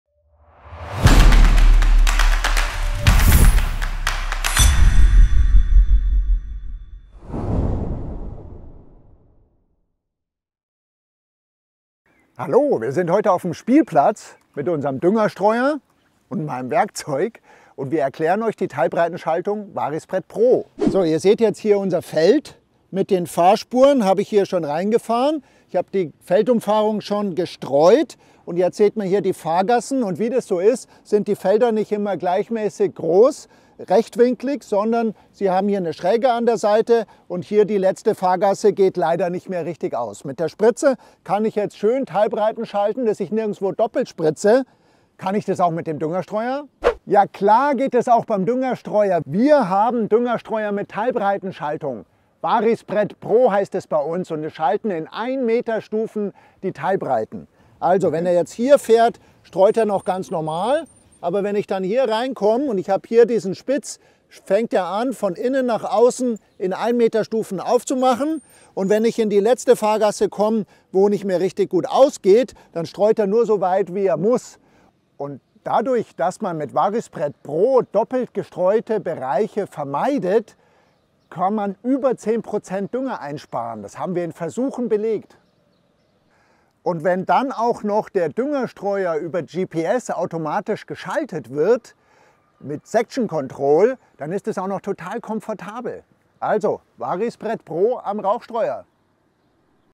VariSpread pro - Einfach erklärt!
🚜 Volker Rathmer erklärt den VarioSpread Pro – Perfektion in der Düngerausbringung! 🌱

In diesem Video zeigt Dir Volker Rathmer anschaulich anhand eines Modells auf dem Spielplatz, wie die innovative Technologie des VarioSpread Pro an unseren AXIS H 30.2/50.2 EMC (+W) und AXIS M 30.2/50.2 EMC (+W) Düngerstreuern funktioniert. Erfahre, wie diese fortschrittliche Funktion die Arbeitsbreite und die Ausbringmenge in einem gleitenden Prozess anpasst, um eine präzise und effiziente Düngerausbringung zu gewährleisten.

Hauptfunktionen von VarioSpread Pro:
✅ Section Control: Software zur stufenlosen Regelung
✅ Randstreuen: Streuen nur im Außenbereich
✅ Mittelstreuen: Streuen über die Mitte hinaus
✅ Automatische Anpassung: Optimale Ausbringung bei nicht aufgehenden Fahrgassen oder Hindernisumfahrungen
✅  Feldumfahrung: Bereits bei der Feldumfahrung wird VariSpread Pro aktiv
✅  SpeedServo-Stellmotoren: Mit den 2,5-mal schnelleren Stellmotoren wird die Düngeeffizienz nochmals verbessert und die Umwelt geschont
✅  Korridorerkennung: VariSpread Pro erkennt unausgestreute Korridore zwischen zwei Fahrgassen und streut diese mit hoher Präzision aus (segmentierte Regelung)

Begleite uns und entdecke, wie VarioSpread Pro die Düngerausbringung revolutioniert und dabei hilft, die Umwelt zu schonen.

Alles über die Düngerstreuer, Salzstreuer und Pneumatikstreuer